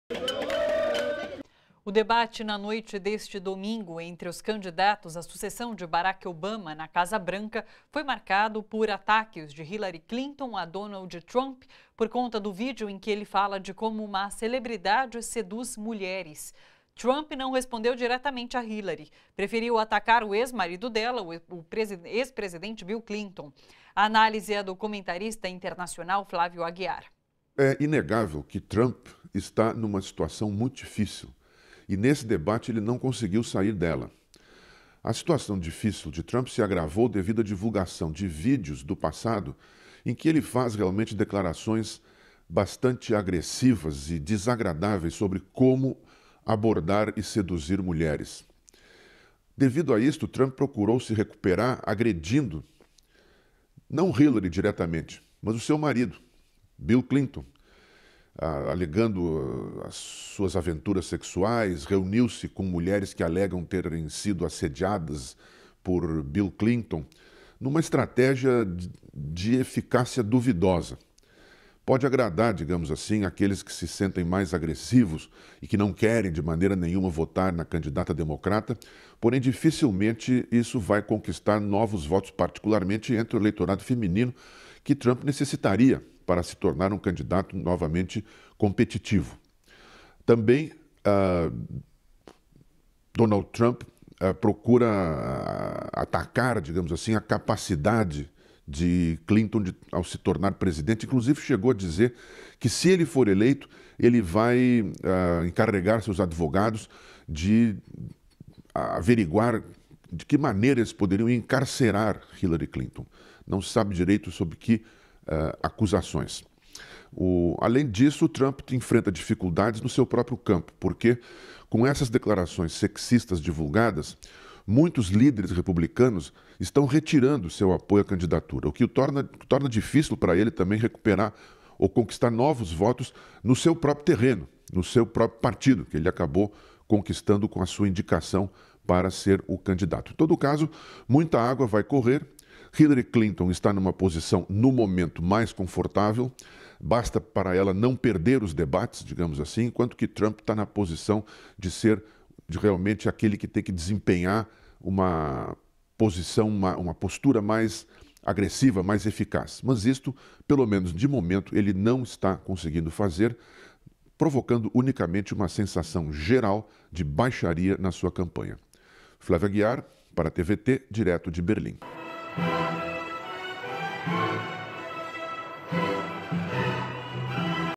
Hillary ataca Trump, que ataca Bill Clinton
O debate de domingo teve ataques de Hillary Clinton a Donald Trump, por conta dos comentários machistas dele em vídeo, mas a resposta foi contra o marido dela, Bill Clinton, analisa o comentarista internacional Flávio Aguiar.

Assine nosso Canal e não perca nenhum conteúdo.